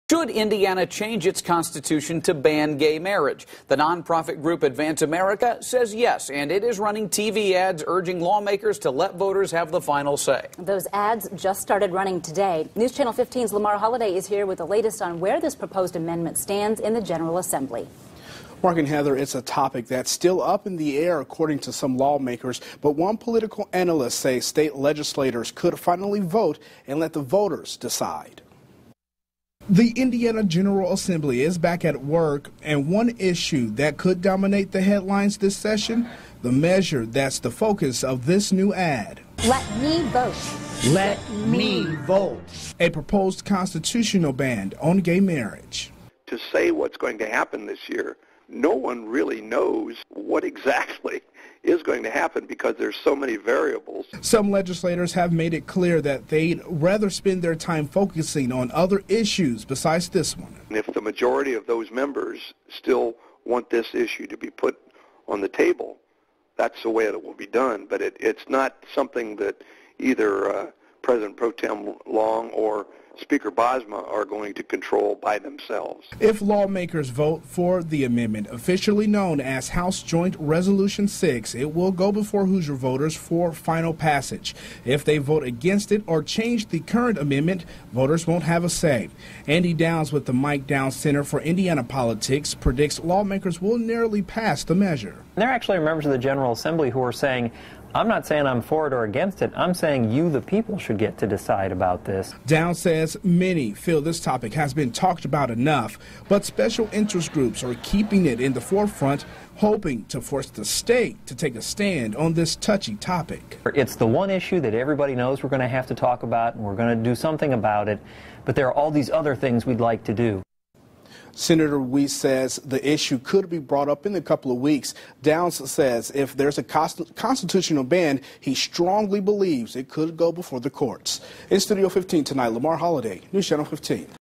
Political interest group releases ad on gay marriage amendment
Advance America released an ad Wednesday to encourage Indiana lawmakers to give the power to the voters to decide on a constitutional ban on gay marriage in the state of Indiana.